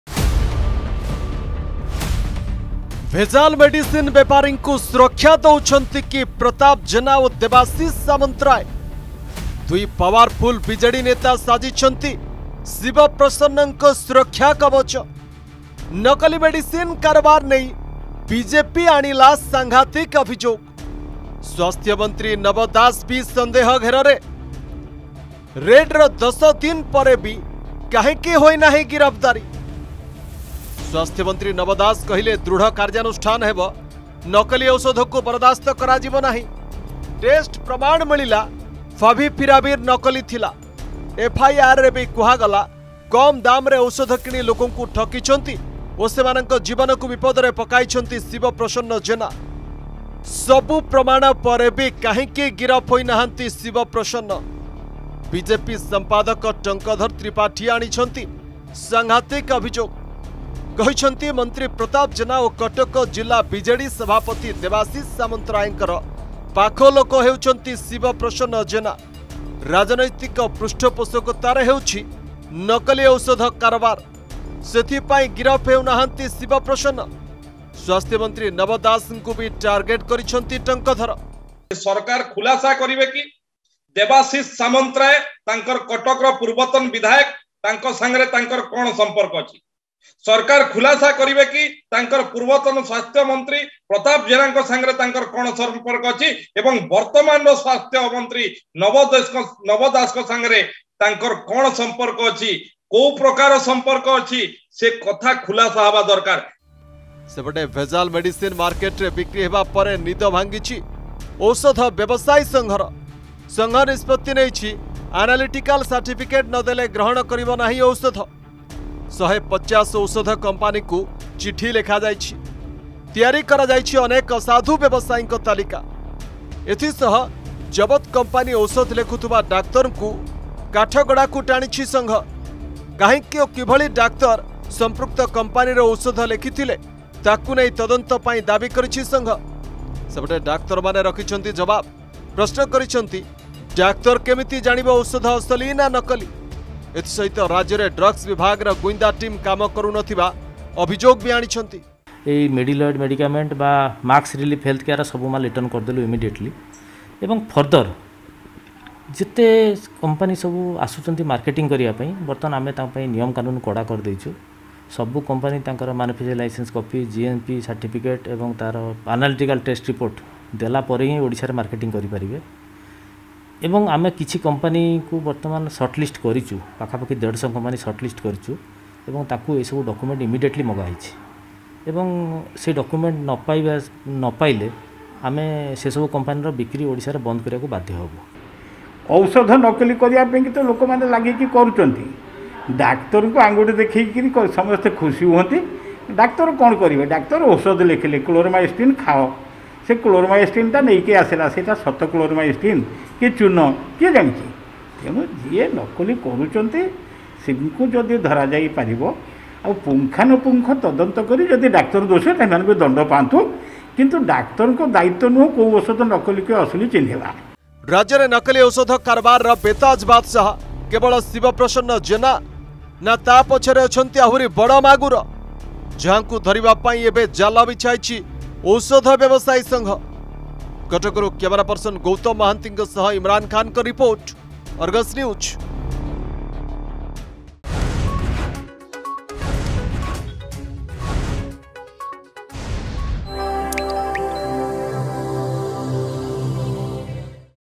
ନକଲି ମେଡିସିନ କାରବାରରେ ବିଜେପି ଆଣିଲା ସାଂଘାତିକ ଅଭିଯୋଗ ।
BJP brings serious allegations against BJD leaders, accuses them of protecting people involved in Corona fake medicines case found in Odisha.
ସ୍ବାସ୍ଥ୍ୟମନ୍ତ୍ରୀ ନବ ଦାସ କହିଲେ ଦୃଢ କାର୍ଯାନୁଷ୍ଠାନ ହେବ, ନକଲି ଔଷଧକୁ ବରଦାସ୍ତ ହେବନାହିଁ। ଏଫଆଇଆର ବି କୁହାଗଲା କମ ଦାମରେ ଔଷଧ କିଣି ଲୋକଙ୍କୁ ଠକିଛନ୍ତି ଓ ସେମାନଙ୍କ ଜୀବନକୁ ବିପଦରେ ପକାଇଛନ୍ତି ଶିବ ପ୍ରସନ୍ନ ଜେନା। ସବୁ ପ୍ରମାଣ ପରେ ବି କାହିଁକି ଗିରଫ ହୋଇନାହାନ୍ତି ଶିବ ପ୍ରସନ୍ନ ଜେନା।